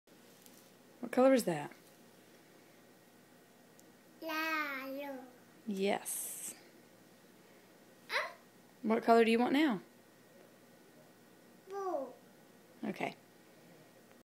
Bren's first art journal
Bren paints with me her mama as the assistant. Learning her colors along with hand eye coordination scooping paint from plate to page. She's the BEST!